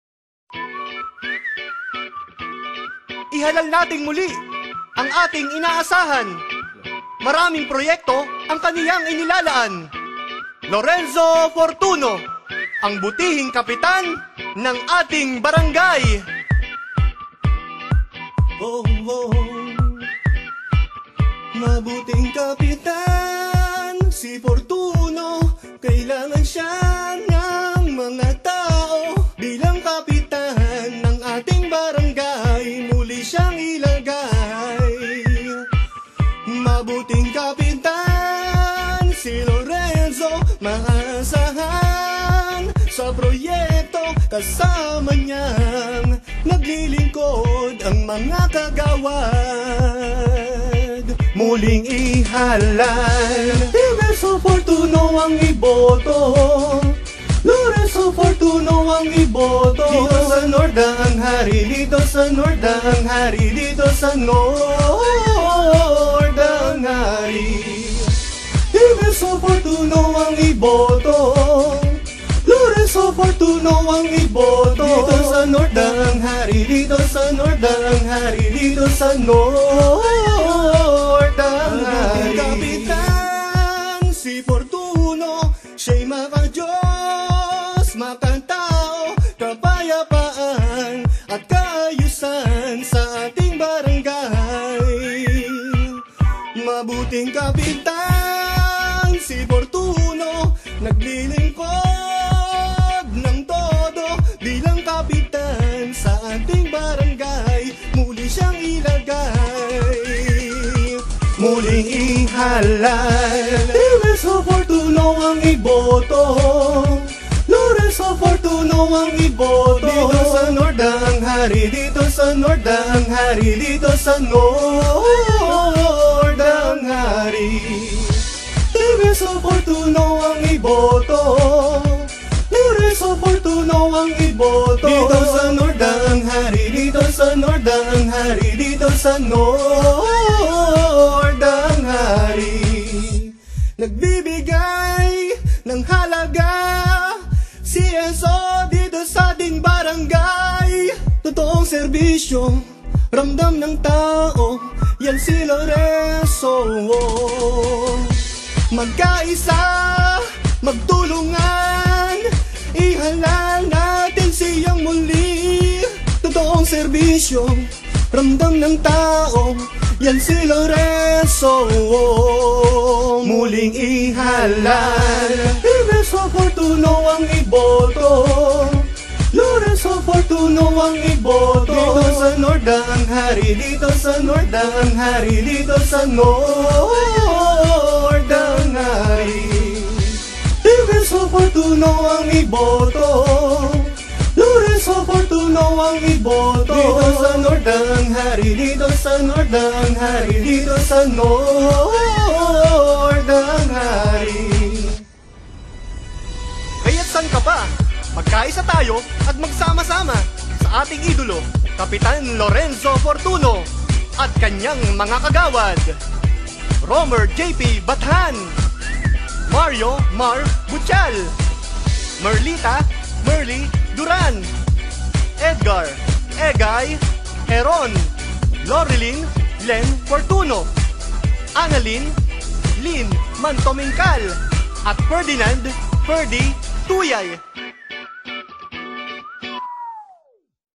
Kapitan Lorenzo Fortuno
Campaign Jingle Sample that I've made... Kapitan Lorenzo Fortuno of North Daang Hari. na nanalong muli bilang Kapitan ng nasabing barangay.. Kindly call me or text me if you would like to have a beautiful campaign jingle like this. For appointment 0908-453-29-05 or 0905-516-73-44